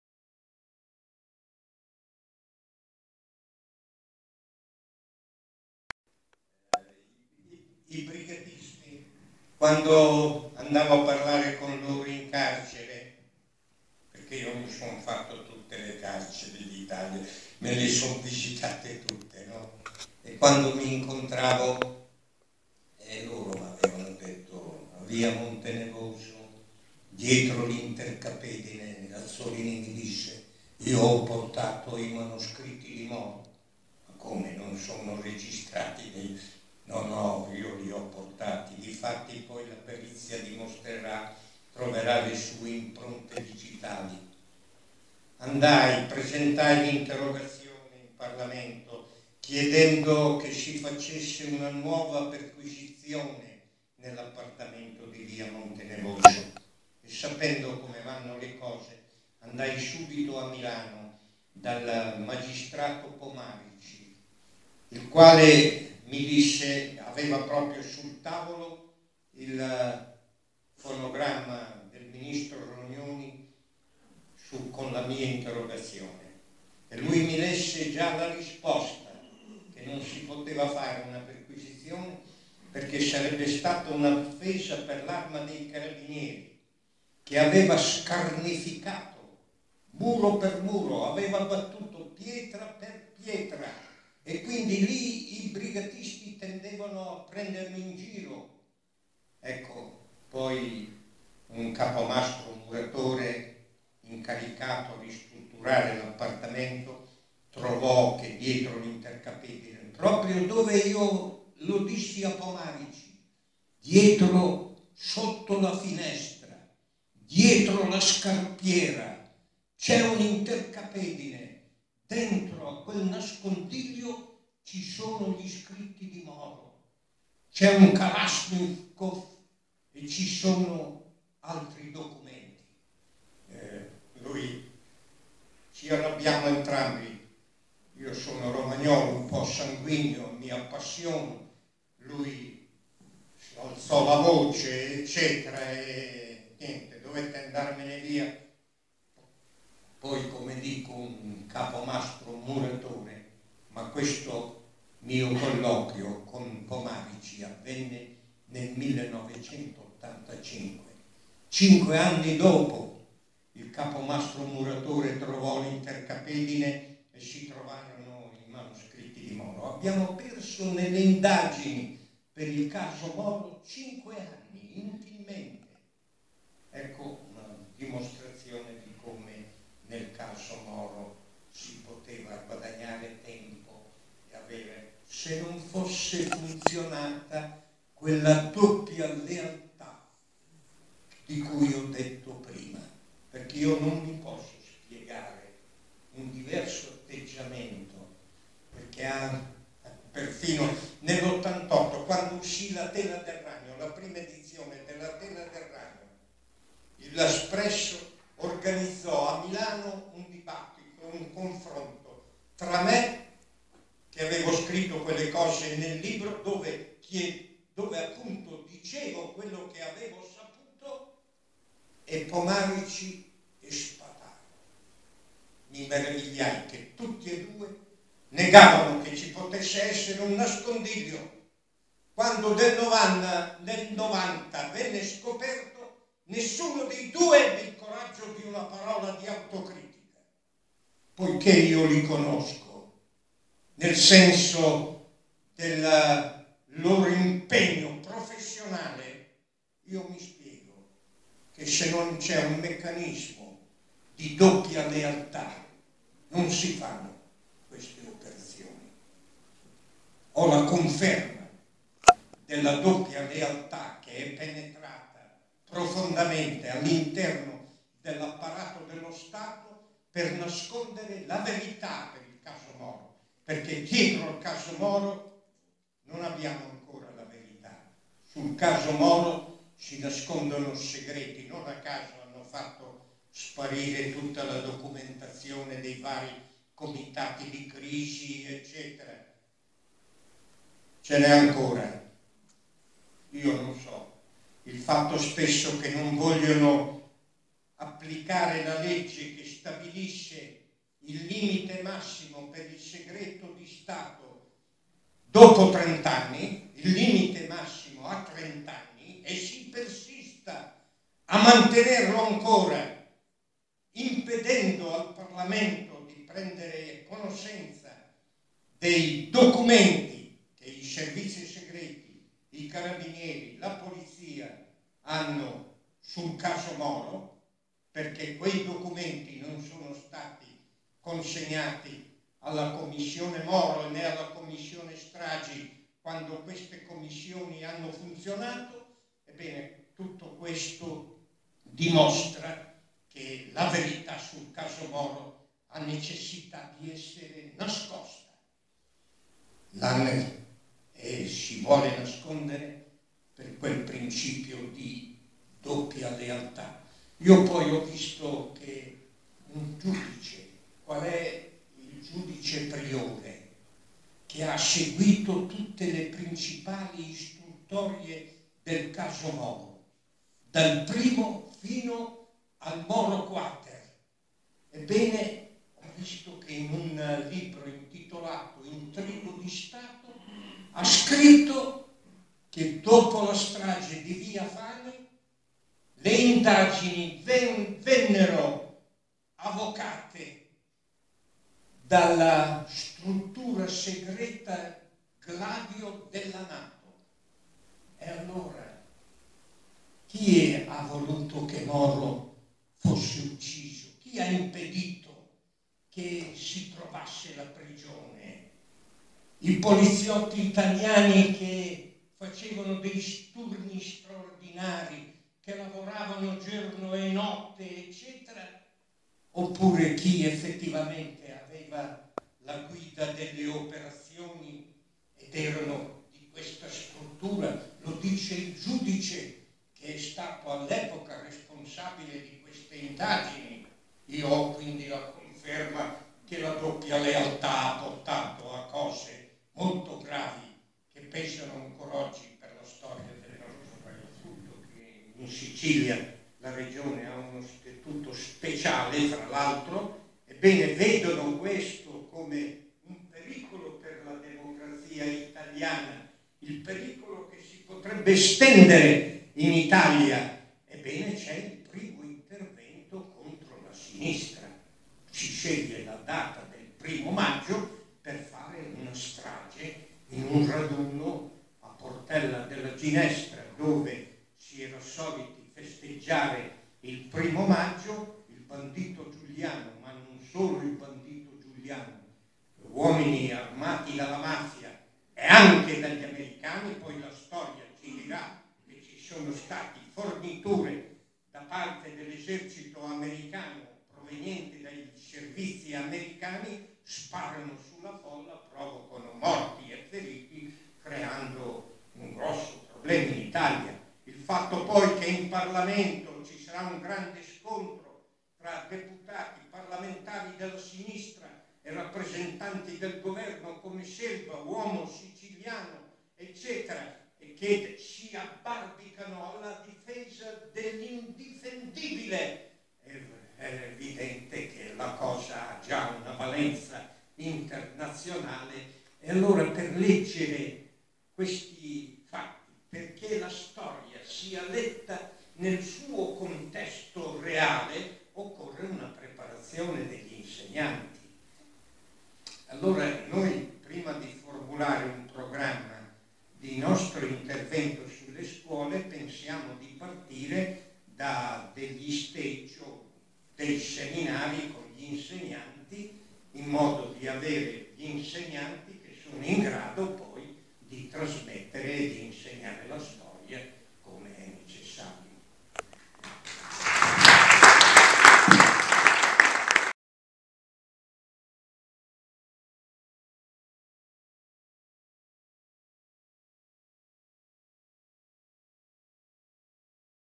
Sergio Flamigni parla della P2 e del caso Moro [4]
Sergio Flamigni viene invitato al Teatro Comunale di Vejano (VT) e parla di cosa è stata e come ha agito la P2 con particolari accenni al caso Aldo Moro. Visuale generale.